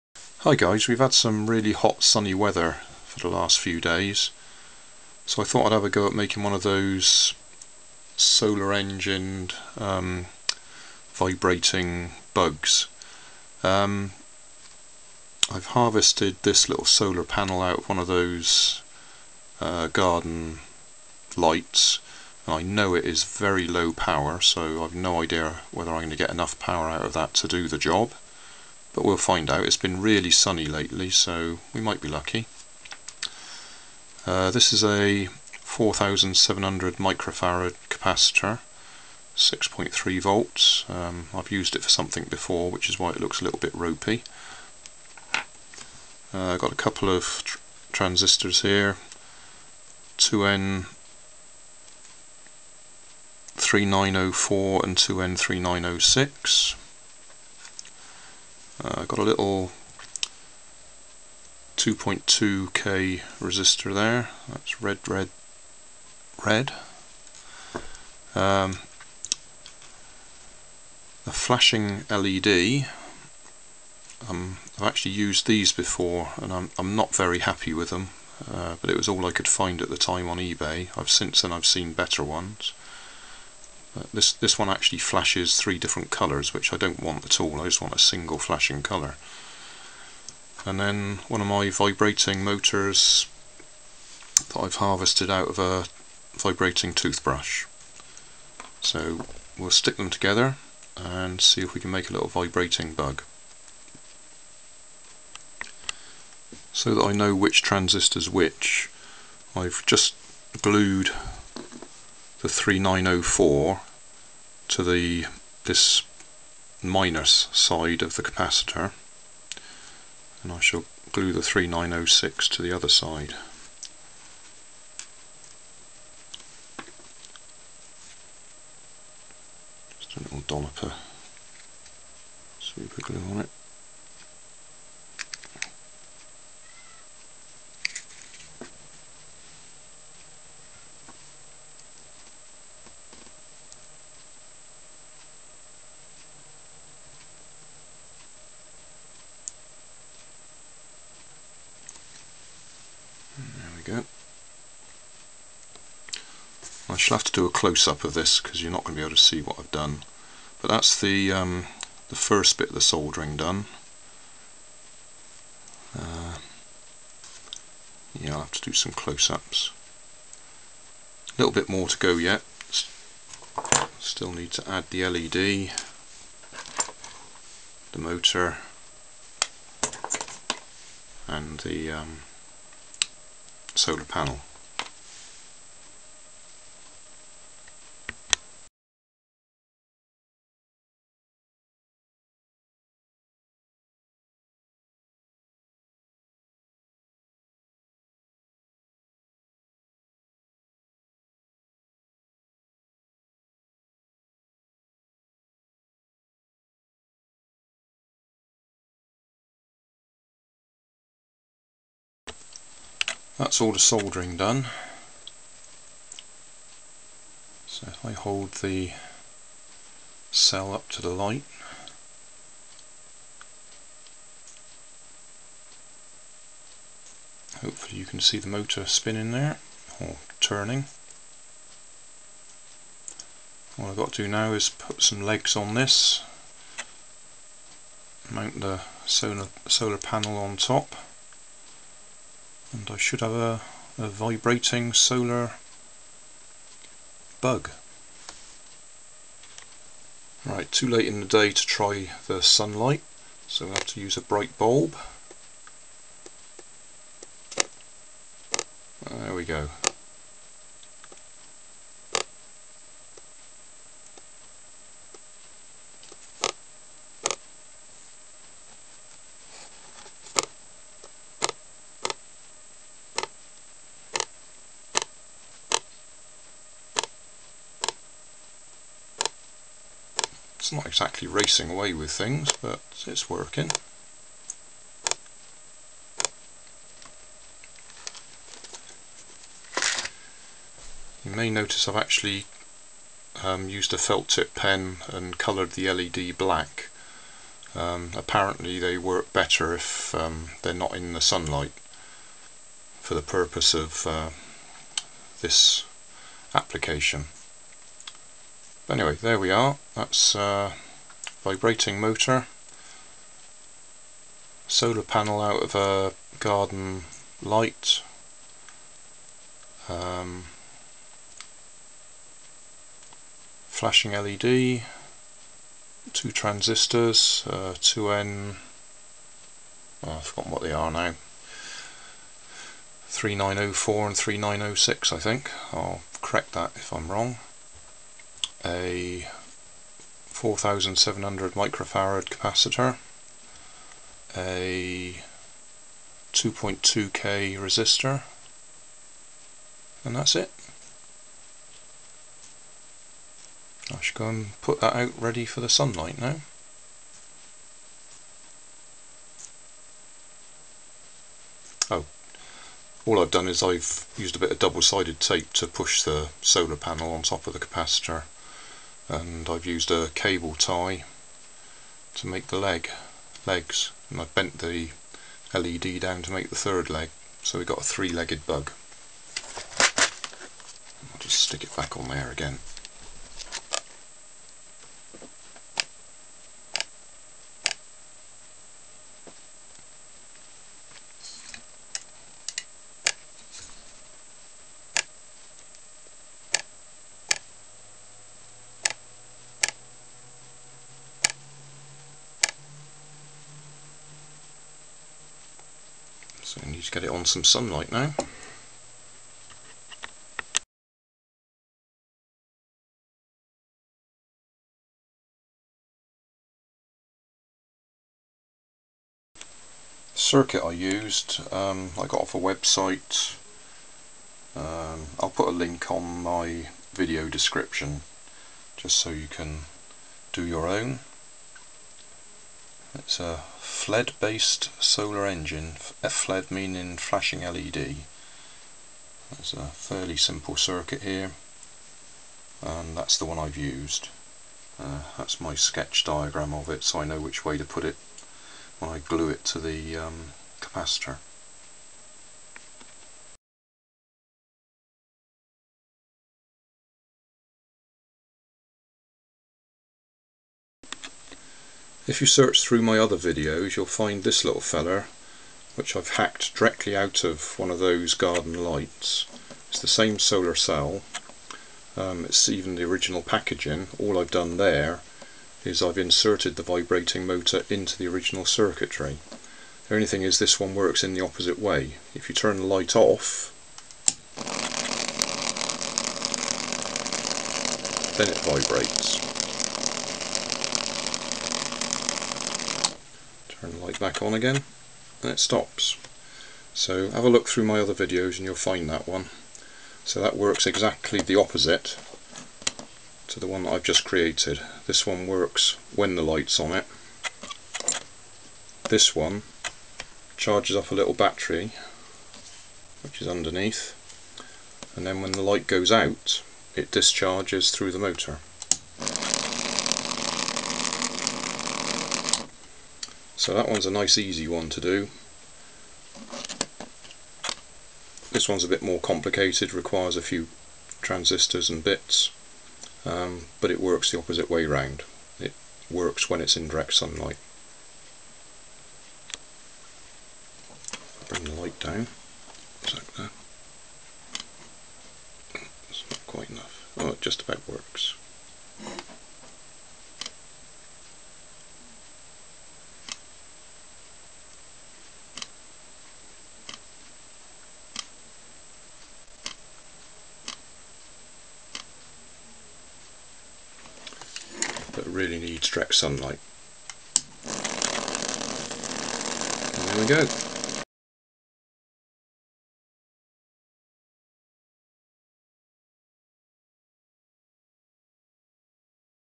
Let's make a Solar Vibrating BEAM Bug
We have had some brilliant sunshine for the last week so my thoughts turned to making one of those solar vibrating bugs. Several months ago I watched an excellent video on how to make a simple one but I have searched for it again and cannot find it so I cannot credit the guy who inspired this project for me. If I do find it again I will add a link.

I did still have the solarbotics.net link that he linked to during his video so I have included that below.

Parts list
Solar Cell - I took mine from a garden solar light
Vibrating Electric Motor - I took mine from a vibrating toothbrush
1 x 2N3904 transistor
1 x 2N3906 transistor
1 x 2.2K resistor
1 x 4700uF Capacitor